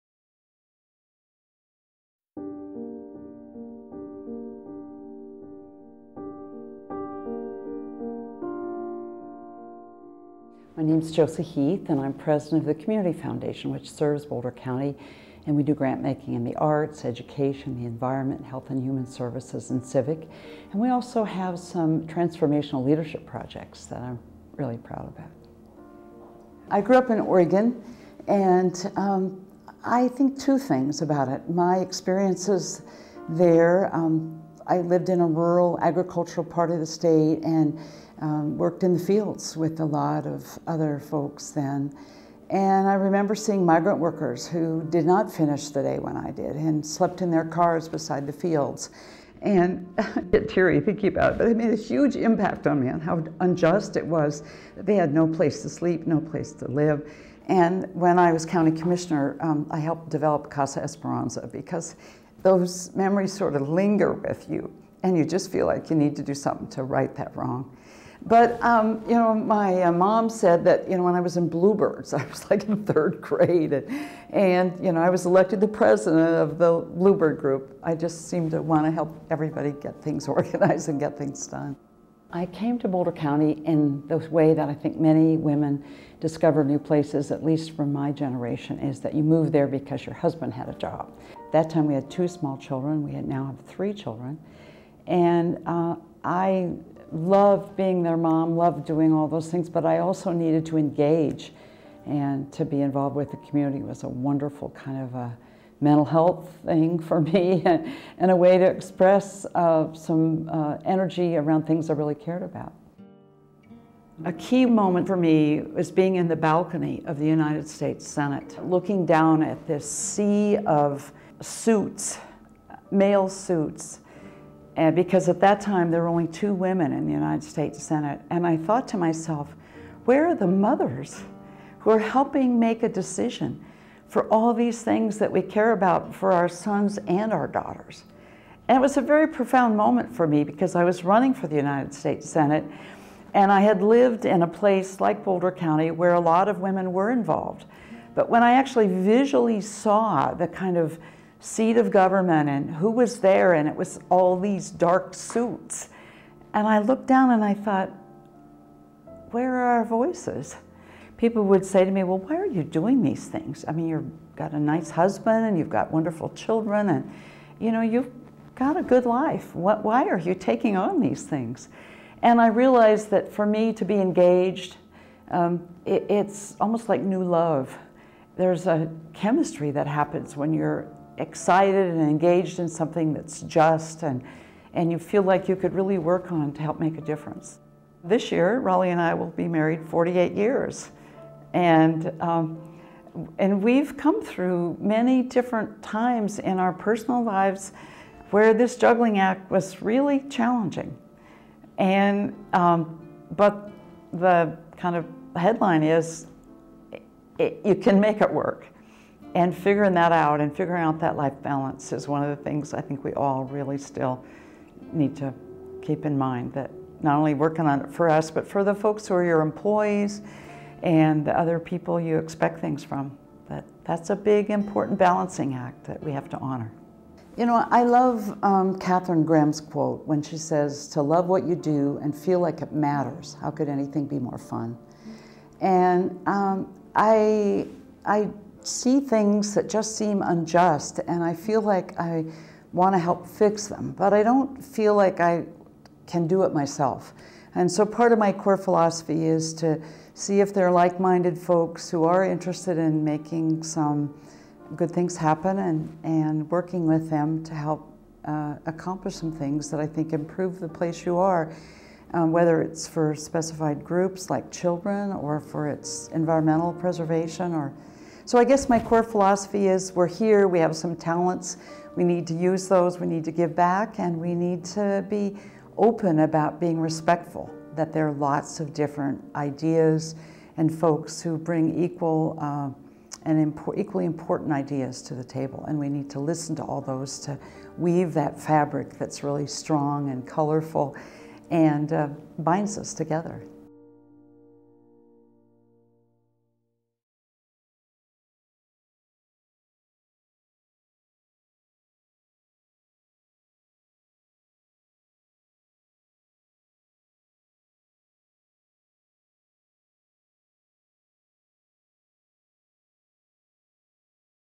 Josie Heath | 2009 Women Who Light The Community Honoree
- Women Who Light the Community, Lifetime Achievement Award. September 18, 2009 in Boulder, Colorado. Video production by Cori Chavez, LLC.